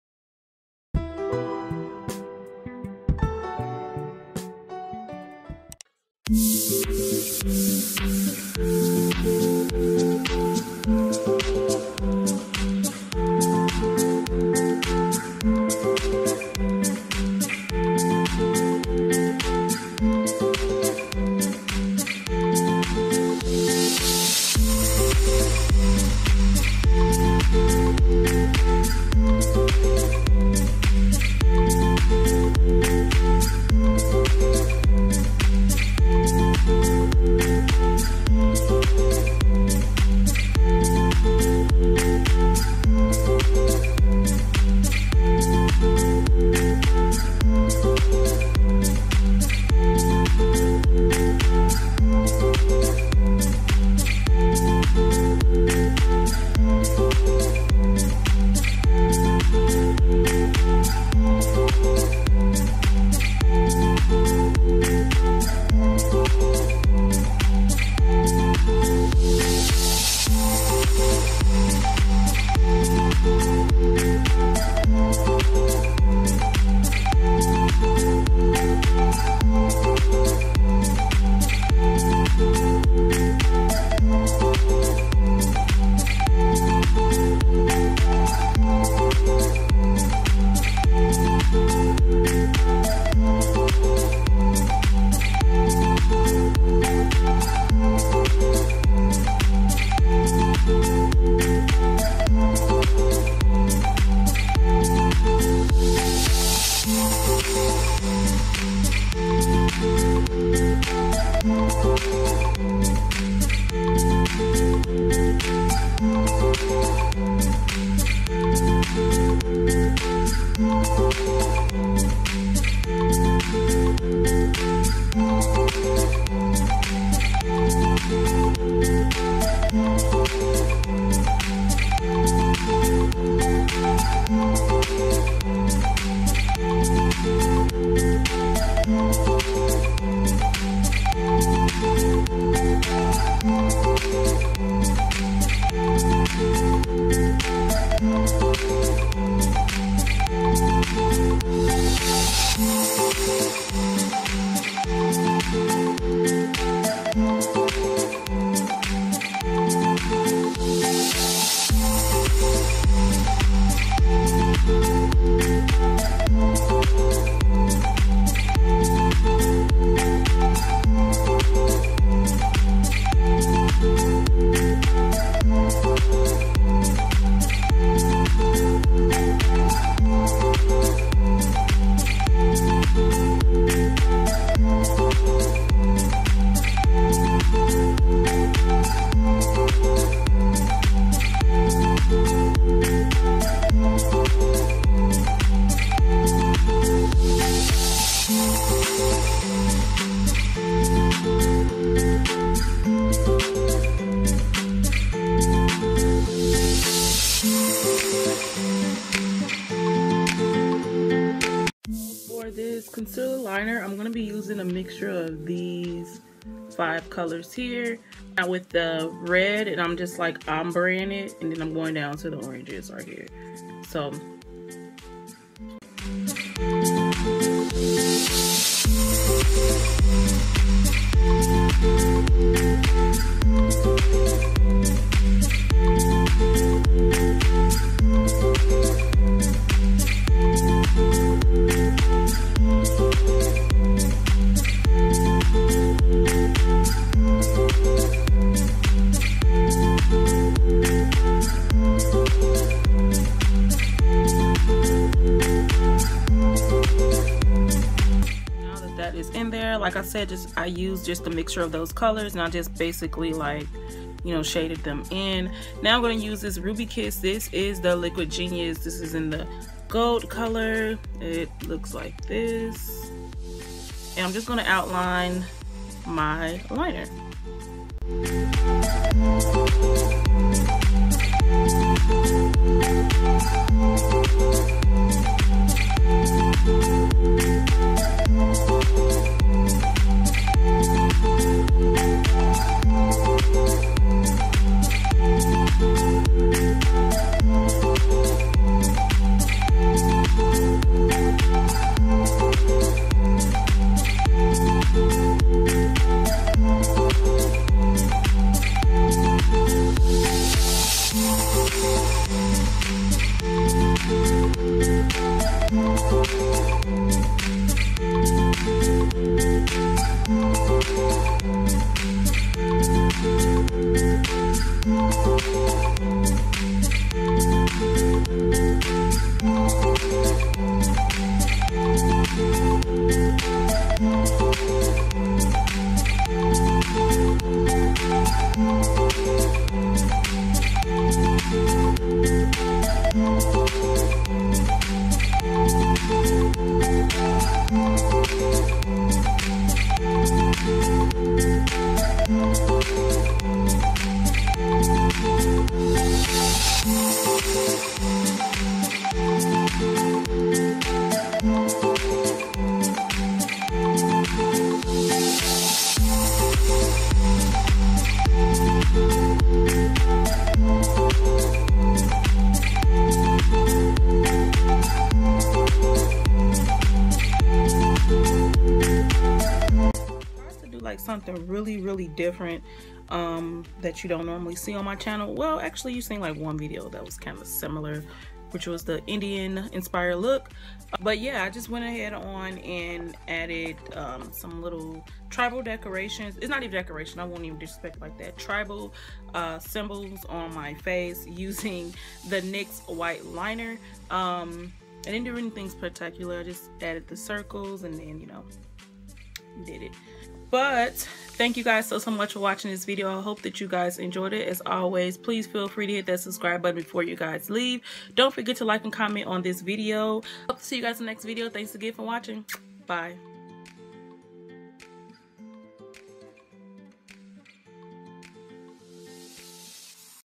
Tribal Bold Eye look | Summertime looks | 2018
Thank you guys so much for watching this video.

I'm Sorry I didn't film me creating the tribal symbols I got a little ahead of myself and forgot to record it lol. But no worries the video I link in the video will go into depth of how I created the symbols so be sure to check that out as well as my foundation routine.

Eyes:
Bh cosmetics- take me back to brazil
Crown " 28 color smoke me out" palette
phsycian formula black gel liner
nyx white gel liner

concealer "warm sand" (cut crease and to conceal my eyes)

face:
milani "golden toffee'
black radiance "brownie"
pro concealer "chesnut"
j.cat setting powder "porcelain"
physician formula bronzer booster
black radiance " black coffee"
milani setting spray "Make it last & Make it dewy"
milani "berry amore" & loreal "brilliant berry" blushes
profusion highlight palette.

Hab. 2:2 (KJV)

SC: Syhilove